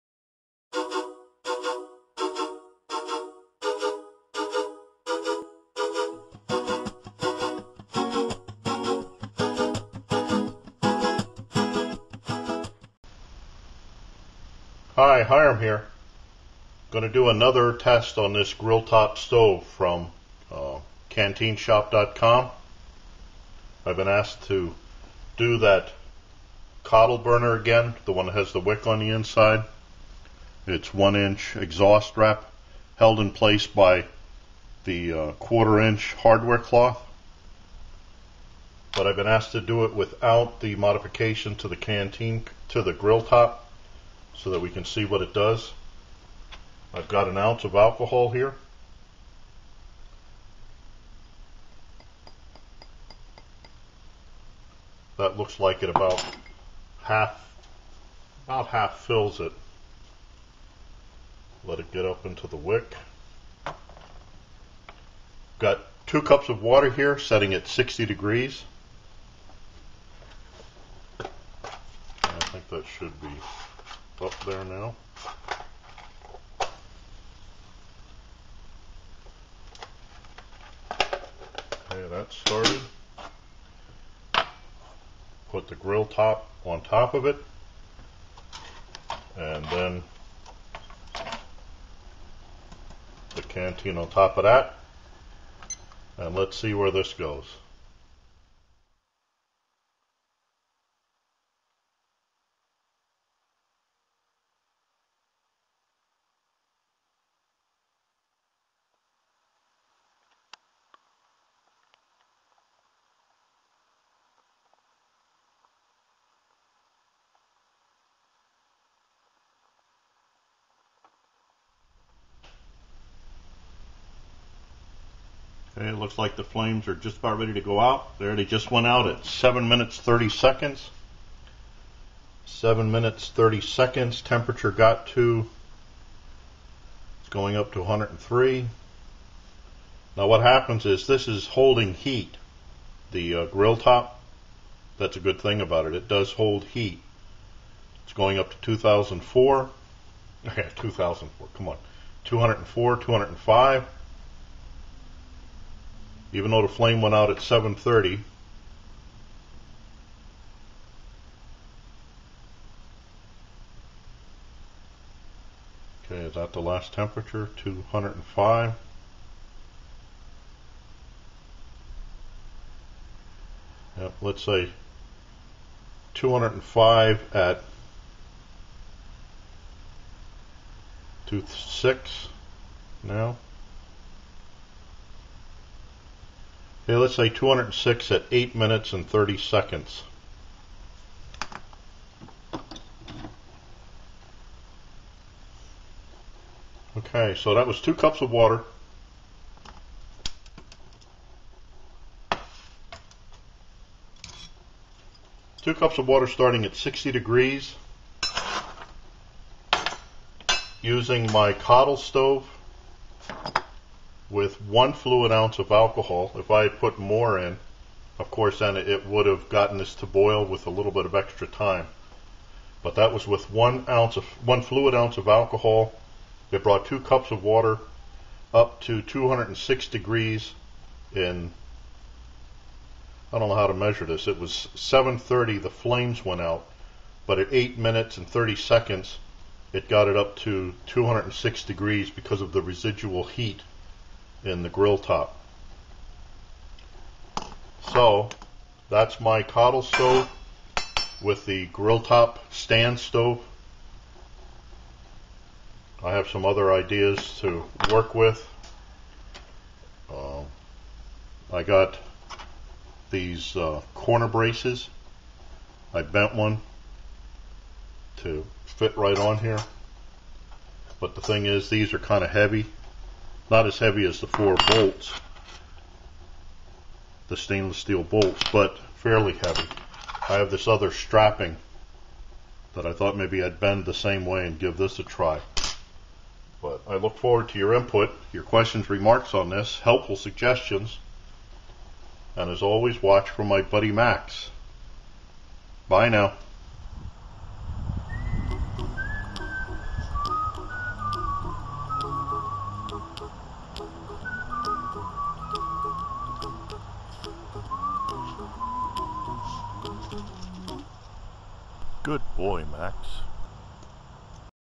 CanteenShop GrillTop Stove Stand - cottle w/ wick burner - Boil Test #8
In this video I use my old Cottle with wick burner with one fluid ounce of denatured alcohol in my CanteenShop GrillTop Stove Stand but instead of using 4 stainless steel bolts and nuts to elevate canteen cup like I did in Lab0172 this time the canteen cup was put right on the GrillTop.

Links:
CanteenShop.com

Lab0174
Title: CanteenShop GrillTop Stove Stand - cottle w/ wick burner - Boil Test #8
Room temp: 56°
Humidity: 75%
Weather condition: Raining
Burner: cottle w/ wick
Stove/Pot stand: CanteenShop GrillTop Stove Stand ( no bolts to elevate canteen cup )
Pot: Canteen Cup - SS - aluminum foil cover
Fuel: Denatured alcohol
Amount of fuel: 1 fluid ounces
Amount of water: 2 cup(s)
Water temp at start: 60°
Time to boil : NA
Time to run-out: 7 minute(s) 30 second(s)
Water temp at run-out: 206° @ 8 minute(s) 30 second(s)
Note: The water temperature kept climbing even after the flame went out. I guess from residual heat in the GrillTop.